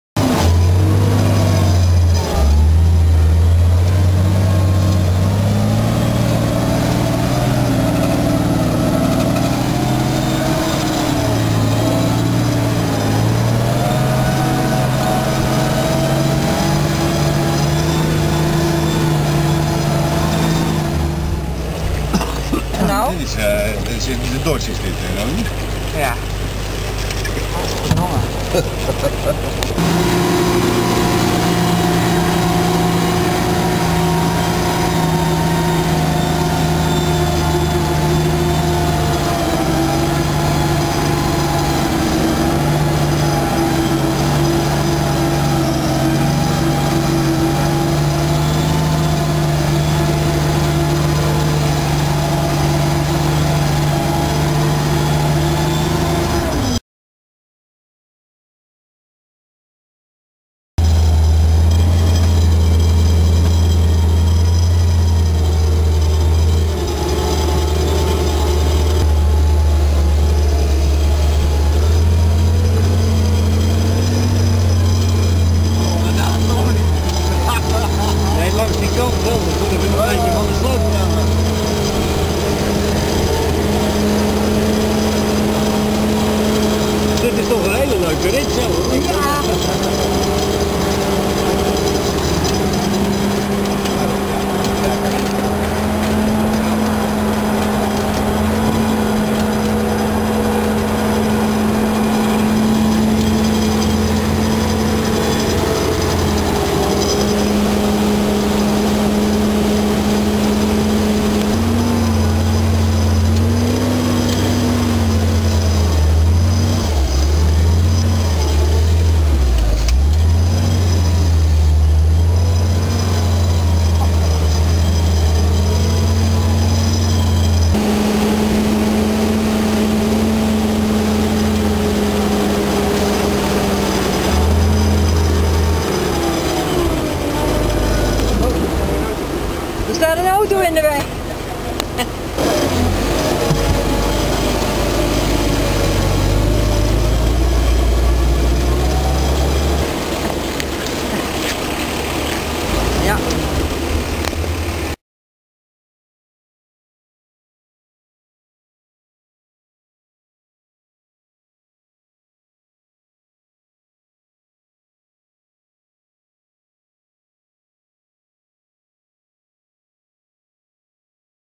Groesezandvreter
14 juni 2009. Na de Groesepapvretersrit met stralend weer, mochten wij na een nacht heftige regen, ook nog even zandhappen in de modder natuurlijk. Dat hebben wij geweten. Dankzij de bereidzaamheid van de chauffeur van de trekker zijn wij er heelhuids uitgekomen. Na een kop koffie konden wij zonder kleerscheuren, maar met heel modderige kleren, weer naar huis rijden.